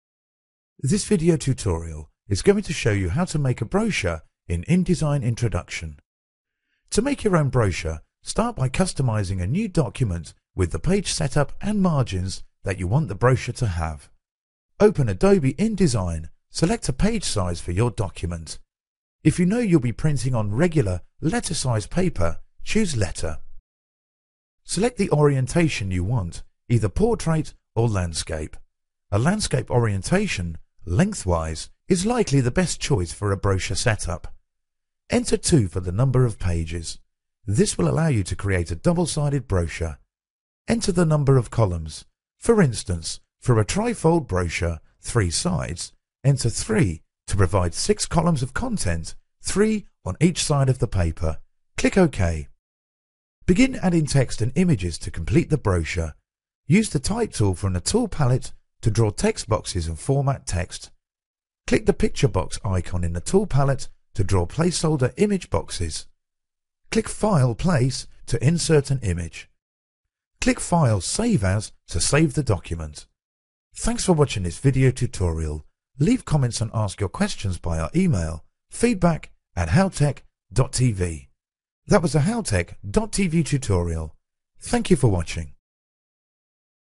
How to Make a Brochure in Adobe InDesign
This video tutorial is going to show you how to make a brochure in InDesign.

You can make your own informational or marketing brochures in inDesign. Start by customizing a new document with the page setup and margins that you want the brochure to have.

Step#1 Open Adobe inDesign

Open Adobe InDesign. Select a page size for your document. If you know you'll be printing on regular, letter-size paper, choose "Letter."

Step#2 Choose the file parameters

Select the orientation you want---either "Portrait" or "Landscape." A landscape orientation (length-wise) is likely the best choice for a brochure setup.

Step#3 Enter '2' for the number of pages
Enter "2" for the number of pages. This will allow you to create a double-sided brochure. Enter the number of columns. For instance, for a tri-fold brochure (3 sides), enter "3" to provide 6 columns of content (3 on each side of the paper). Click "OK.

Step#4 begin adding text and images to complete

Begin adding text and images to complete the brochure. Use the "Type" tool from the tool palette to draw text boxes and format text. Click the picture box icon in the tool palette to draw placeholder image boxes. Click "File, Place" to insert an image.

Step#5 click file and 'save as'

Click "File, Save as" to save the document.